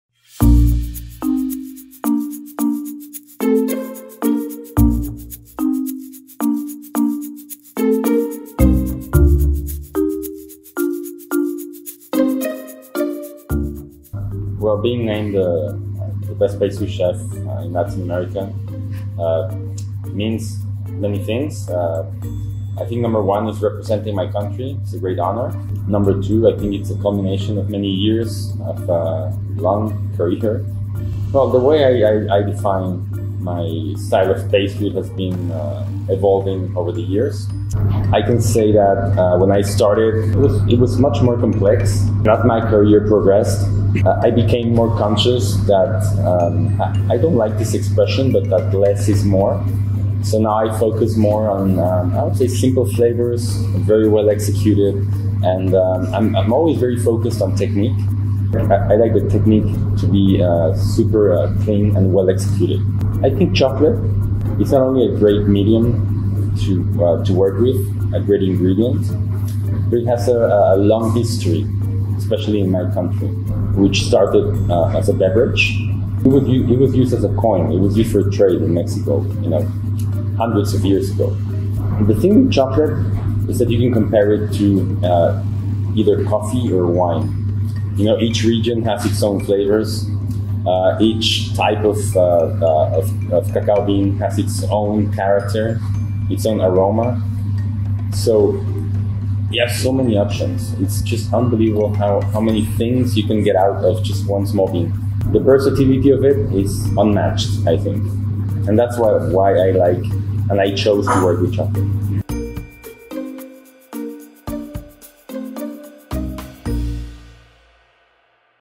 Meet Luis Robledo: The Best Pastry Chef In Latin America
Discover more about the award winning chef in this exclusive interview with Latin America's 50 Best Restaurants.